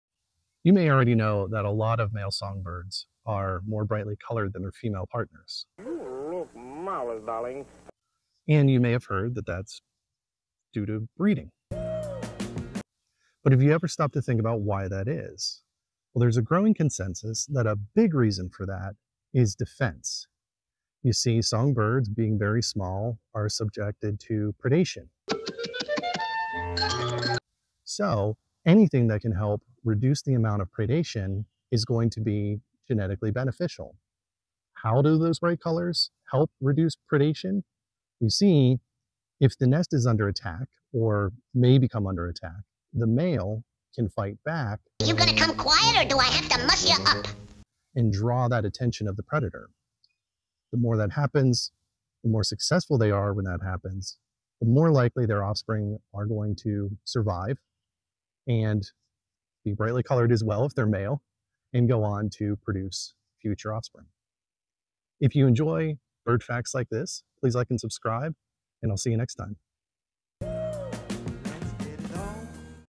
The reason male birds are often more colorful might surprise you.
Male birds are often more colorful than their female partners. You may already knows this but do you know why?

Being more brightly colored does often winover partners for male birds. However, there's a growing consensus that this bright showiness is helpful for defending their partners and offspring in the nest.

By drawing away predators the offspring of colorful male birds have a greater chance to survive and to also reproduce.